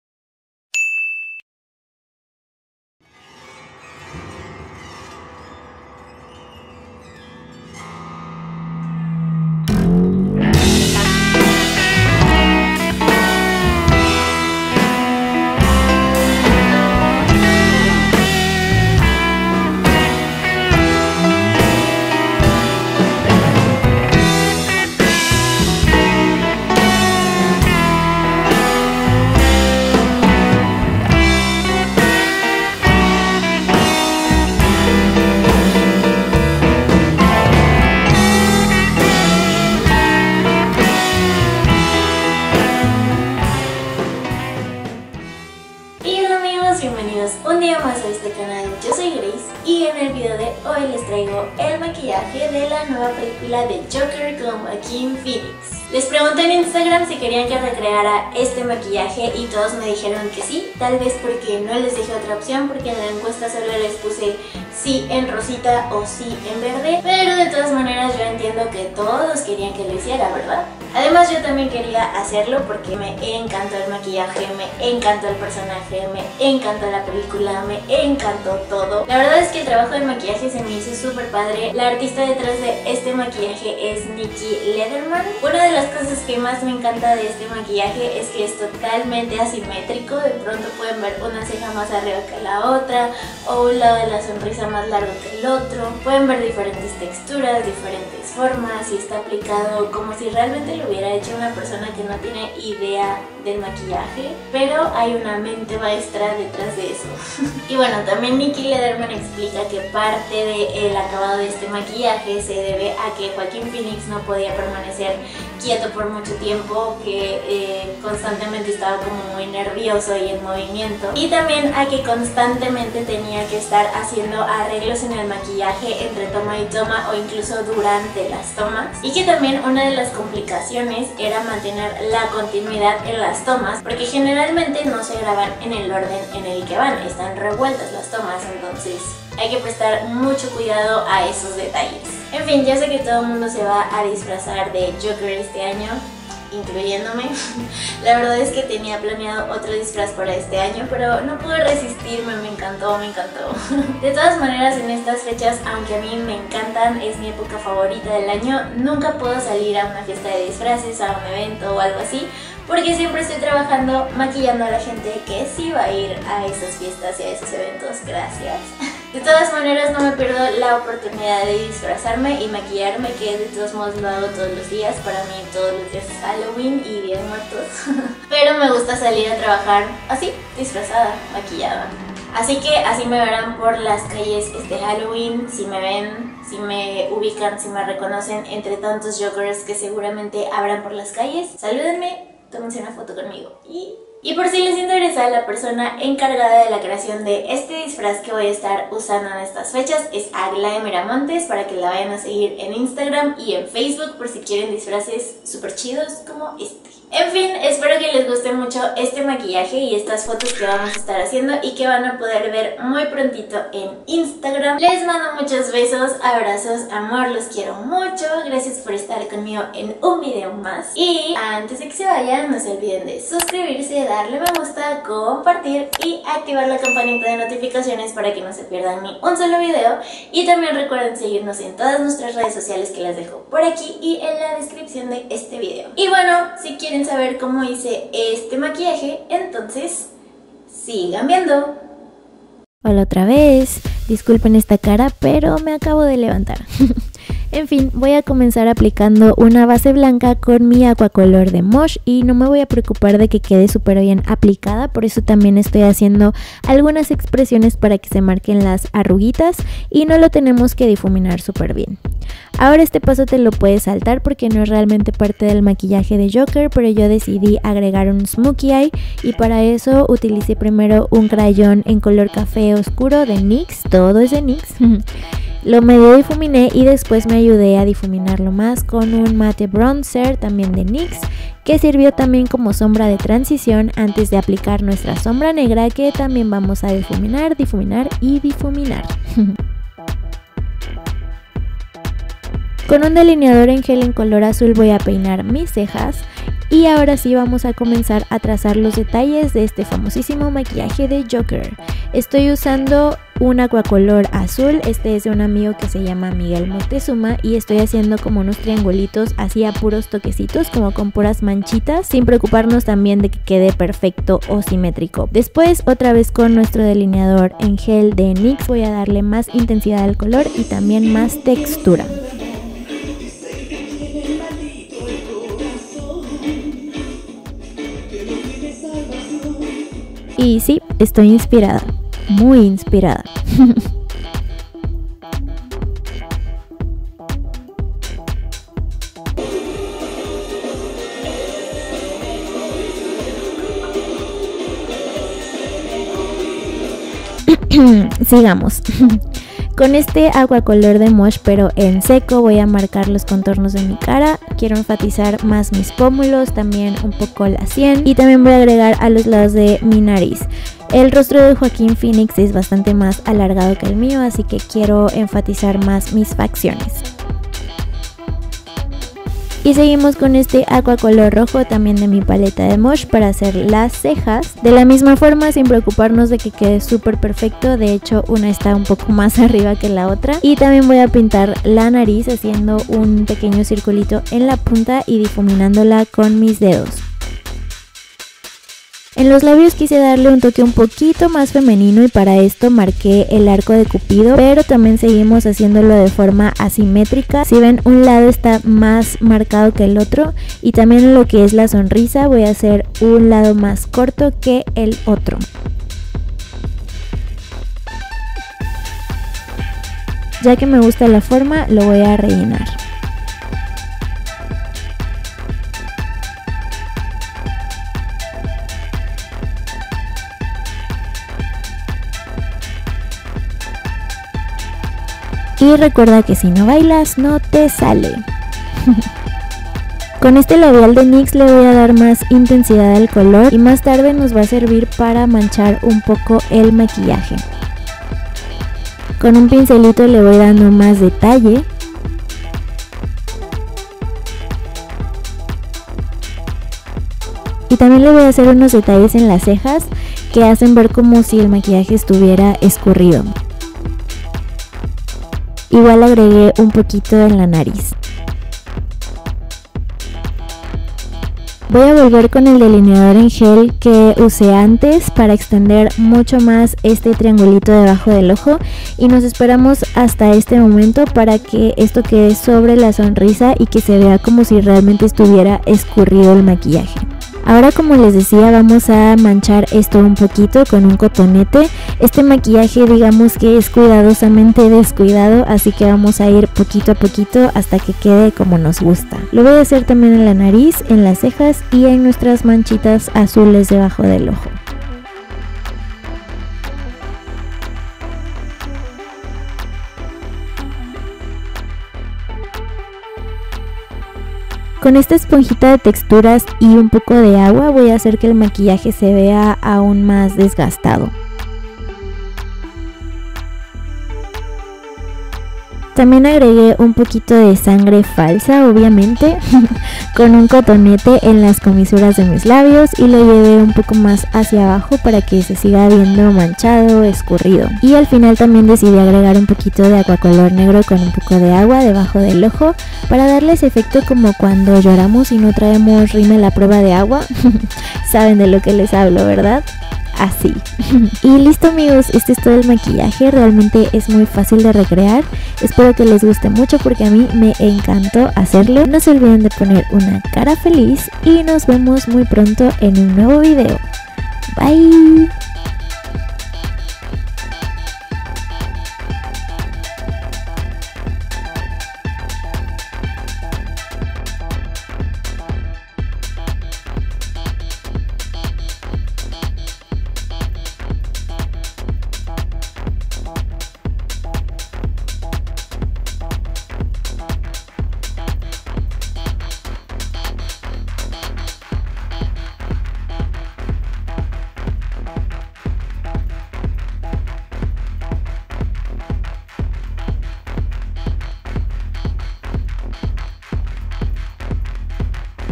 JOAQUIN PHOENIX JOKER MAKEUP | HALLOWEEN
"I used to think my life was a tragedy. But now I realize, It´s a comedy."

Hey hola amigos!! Antes que nada HAPPY HALLOWEEN! Toda esta temporada de Halloween - Día de Muertos es mi temporada favorita del año, aún más que mi cumpleaños. Es una locura! Cuéntenme cuales fueron sus atuendos y maquillajes para celebrar...

Espero que les guste esta interpretación de la nueva película de Joker con Joaquin Phoenix. Me encantó la película, el maquillaje, el personaje, TODO! Además de que Joaquin Phoenix es un super activista por los derechos de los animales, vegano, y ha apoyado un par de documentales buenísimos sobre este tema. No solo soy su fan por su actuación sino por toda su labor en favor de los animales.

En fin, les mando muchos besos, abrazos y amor y espero que disfruten muchísimo estas fechas, que se disfracen que se dviertan, que vayan por muchos dulces, pero también que pongan sus ofrendas bien bonitas para recibir a los que cada año vuelven a este mucho a vistarnos. Nos vemos muy pronto en un nuevo vídeo!

MAKEUP ART & EDICIÓN
Graziella Montagner | Grace

PHOTO & VIDEO INTRO
Rocko Von Droms

VESTUARIO
Aglae Miramontes

CONTÁCTANOS!